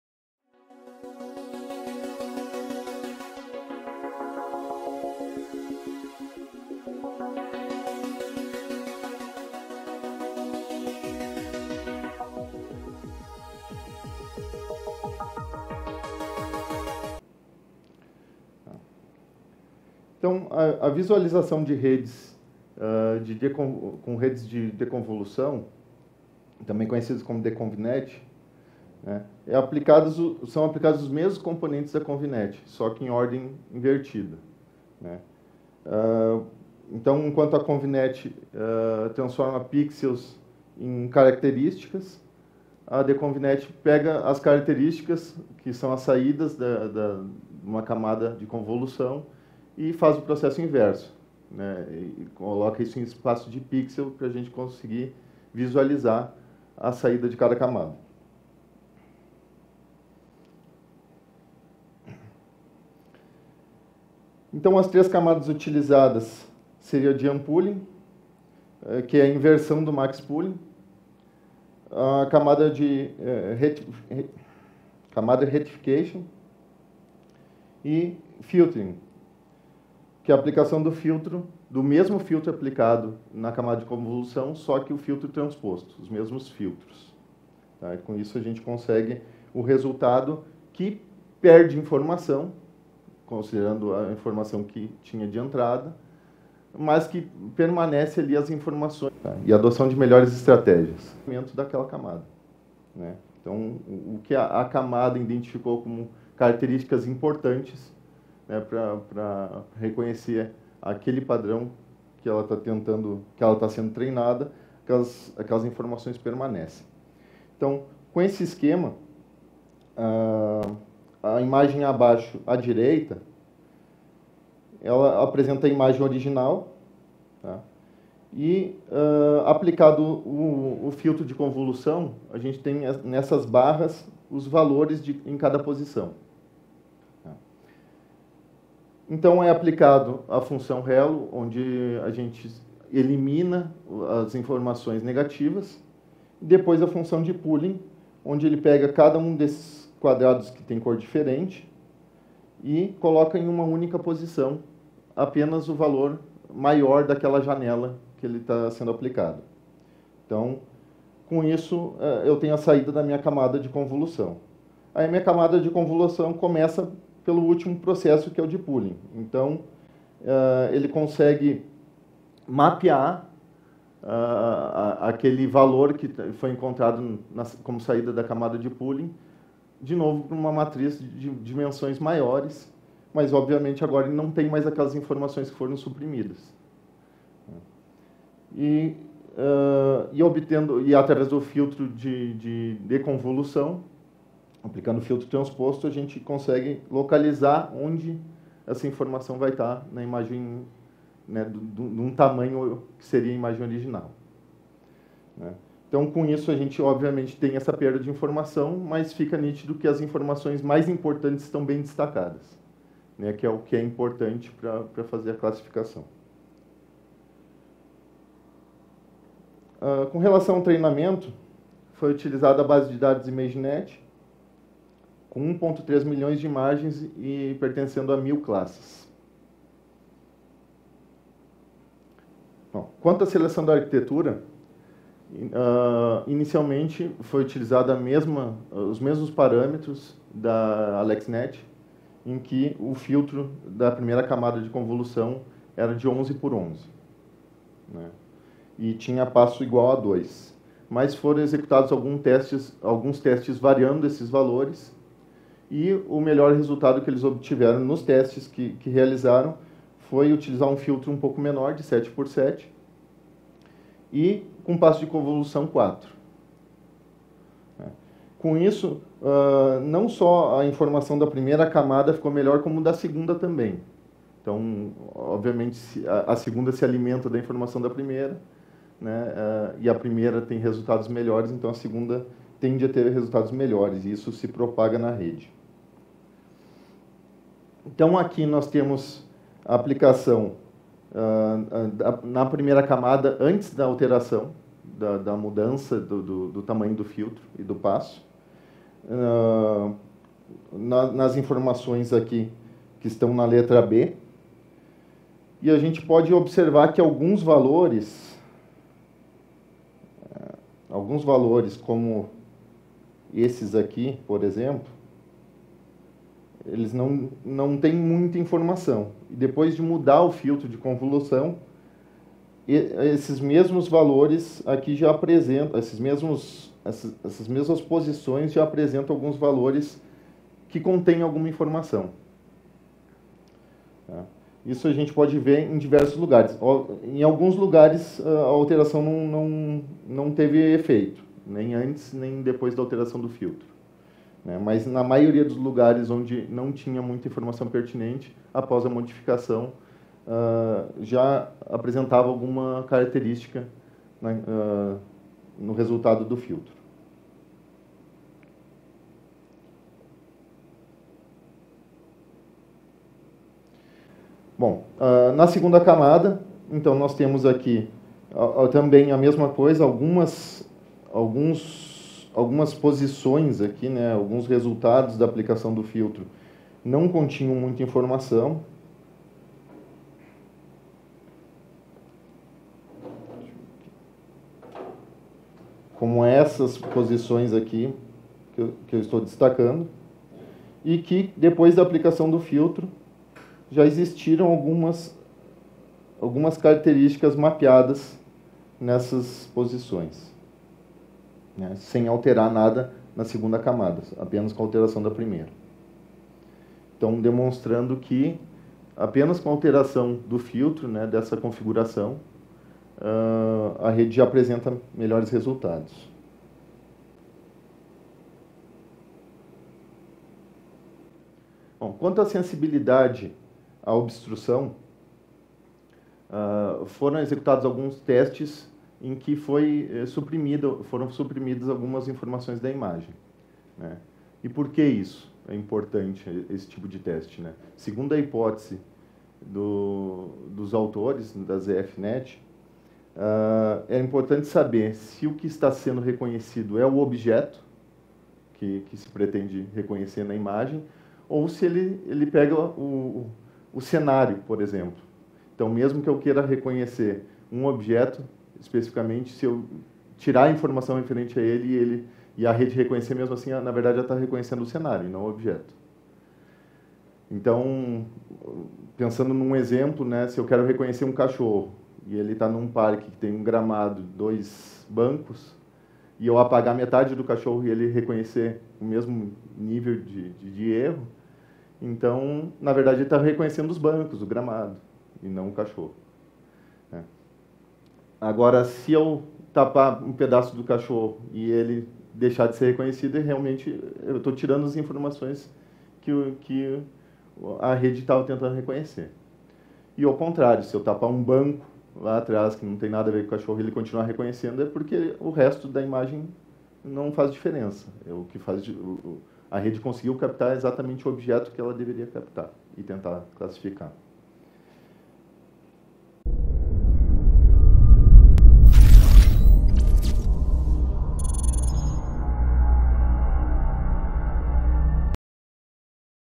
ZFNet - Parte II
Visualizando e entendendo as camadas convolucionais em redes neurais profundas.